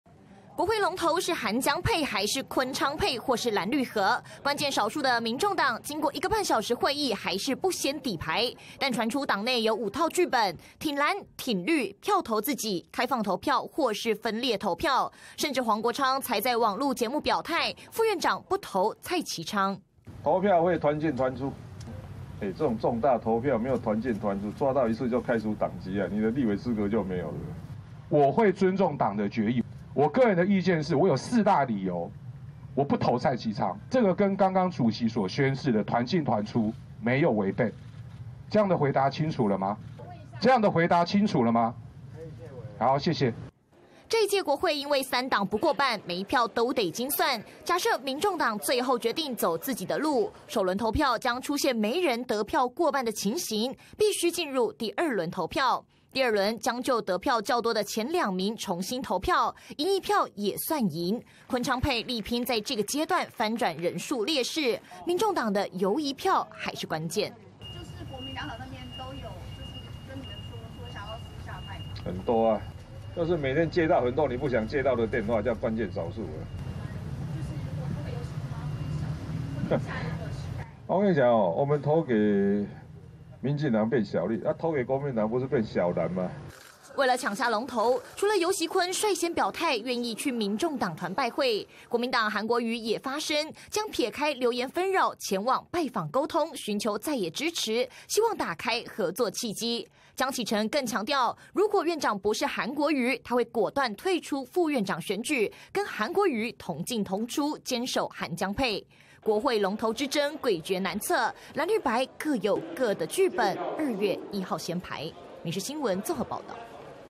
立法院長投票「民眾黨團進團出」 柯文哲：跑票開除黨籍－民視新聞
【民視即時新聞】國會龍頭之爭，根據民眾黨八位立委的投票意向，有各種劇本，但柯文哲下令，投票一定團進團出，否則被抓到直接開除黨籍，不分區也不用當了！因為像是黃國昌，前一天才在網路節目上說自己不投蔡其昌，被問到團進團出，他爆氣回應，會尊重黨的決議，還連問三次，這樣的回答清楚了嗎？